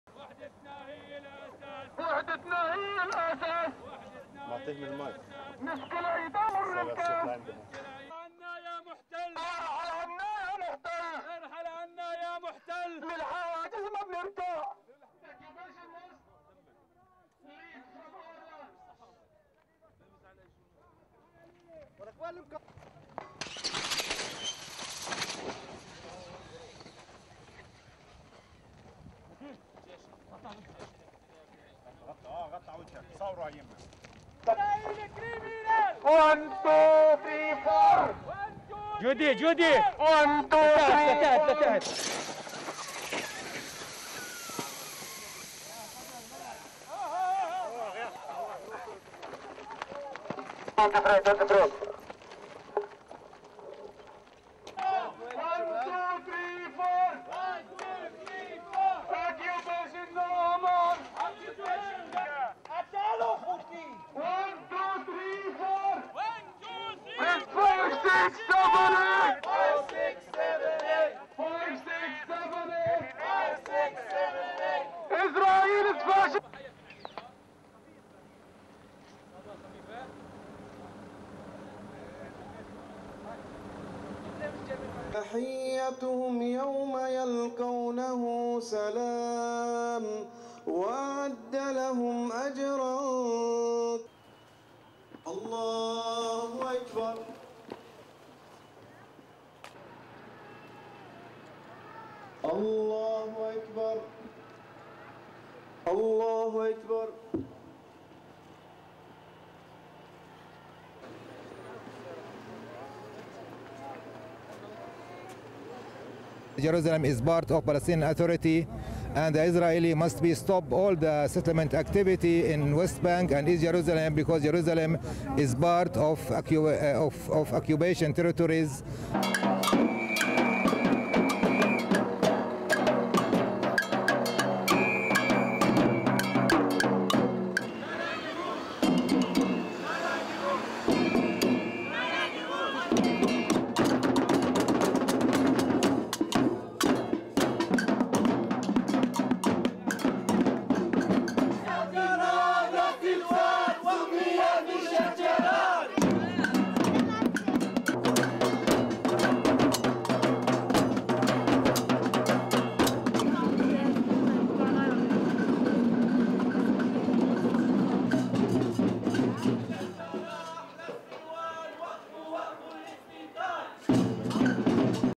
Demonstration against separation barrier; settlements
Bilin, West Bank
1. Wide of protesters marching towards barrier carrying Palestinian flags and banners
2. Mid of marchers waving flags
3. Mid of Israeli security forces behind barrier
4. Tear gas being fired over fence, landing near protesters
5. Man with bag on his head to prevent effect of tear gas
6. Tear gas billowing around protesters
7. Demonstrator attempting to cut barbed wire
8. Palestinian picking up rock
9. Wide of protest
10. Mid of Israeli security forces
11. Wide of demo
Jerusalem
12. Pan right from men praying to Silwan neighbourhood
13. Men kneeling to pray
14. Wide of men praying
15. Mid of men praying
16. Wide of prayer dispersing
17. SOUNDBITE: (English) Hatem Abdel Qader, Palestinian minister of Jerusalem affairs:
"Jerusalem is part of Palestinian Authority and the Israelis must stop all the settlement activity in West Bank and east Jerusalem because Jerusalem is part of occupation territories."
18. Wide of protest against Jewish settlement in east Jerusalem with signs and instruments
19. Wide of protest with marchers chanting "end to the occupation"
20. Protesters holding signs reading "Free Shakh-Jarah" (neighbourhood in east Jerusalem)
21. Pull out from house where Palestinians have been evicted to protesters
22. Protesters drumming
23. Wide of Israeli police
24. Wide of protest with Israeli police standing watch
STORYLINE
Palestinian protesters and peace activists clashed with Israeli soldiers during their weekly demonstration against Israel's separation barrier in the West Bank village of Bilin on Friday.
The protesters marched towards the barrier and threw stones at Israeli soldiers on the other side.
The soldiers responded by launching tear gas grenades.
Demonstrations have been held every Friday in Bilin, near the West Bank city of Ramallah, for more than three years.
Israel started building the barrier in the West Bank seven years ago amid a deadly wave of suicide bombings.
Israel says the barrier is essential to its security, but Palestinians claim it is a land grab and have been holding weekly protests against the winding barrier.
The 680-kilometre (425-mile) barrier is a combination of concrete walls, fences, trenches and patrol roads.
It runs along the line between Israel and the West Bank but in certain parts cuts into Palestinian territory.
The Palestinians say the barrier severs them from their land, disrupts their lives and cripples their economy.
They have mounted a series of Supreme Court challenges to the barrier's route, forcing Israel to make adjustments.
Other smaller demonstrations took place in Jerusalem on Friday as people protested expansion activity on disputed land on the outskirts of Jerusalem, after Israel broke ground on a new housing complex in Gilo this week.
Dozens of protesters gathered near the neighbourhood of Silwan.
Some of the area's residents face eviction to make room for an archaeological park.
At another demonstration, Israeli activists protested continued settlement activity in the Holy City's eastern part and marched towards Sheikh Jarrah, the predominantly Arab neighbourhood where Palestinians have been evicted by Israeli authorities.
At the time of the eviction, Israeli police cited a ruling by the country's Supreme Court that the houses belonged to Jews and that the Arab families had been living there illegally.
The Palestinians claim the West Bank and east Jerusalem - areas captured by Israel in the 1967 Mideast war - for their hoped-for state and have refused negotiations until Israel stops settlement construction in these areas.